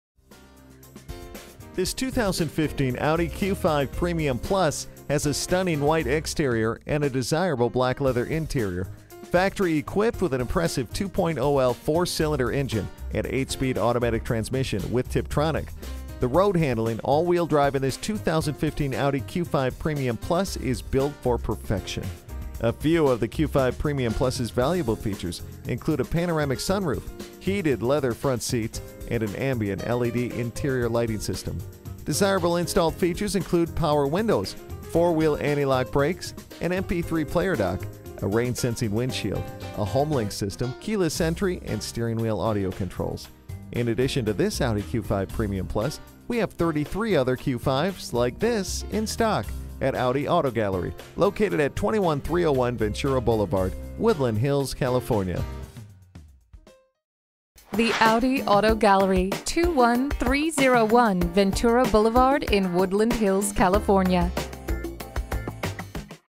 2015 Audi Q5 Los Angeles Woodland Hills, CA NAF124689 - SOLD
Year: 2015
Make: Audi
Model: Q5
Trim: Premium Plus
Engine: 2.0L 4-Cylinder TFSI
Transmission: 8-Speed Automatic with Tiptronic
Color: White
Interior: Black
Stock #: NAF124689
VIN: WA1LFAFP0FA124689
This 2015 Audi Q5 Premium Plus stock # NAF124689 has a stunning white exterior and a desirable black leather interior. Factory equipped with an impressive 2.0l 4-cylinder tfsi engine and 8-speed automatic with tiptronic transmission, the 2015 Audi Q5 Premium Plus produces an impressive 0.0 city MPG / 0.0 HWY MPG. Add in the road handling all wheel drive and this 2015 Audi Q5 Premium Plus is built for perfection. A few of the Q5 Premium Plus's valuable features include: a panoramic sunroof, heated front seats, and an ambient LED interior lighting system. Desirable installed features include: Power Windows, 4-Wheel Anti-Lock Brakes, an MP3 Player Dock, a Rain Sensing Windshield, a Homelink System, Keyless Entry, Steering Wheel Audio Controls, and Leather Seats. In addition to this Audi Q5 Premium Plus We have 33 other Q5's like this in stock at Audi Auto Gallery located at 21301 Ventura Boulevard Woodland Hills, Ca! This 2015 Audi Q5 Premium Plus also comes factory equipped with: an Anti Theft System, Multi-zone Climate Control, Power Mirrors, Integrated Turn Signal Mirrors, Heated Mirrors, a Leather Wrapped Steering Wheel, a Power Drivers Seat, and an Overhead Console.

Address:
21301 Ventura Blvd.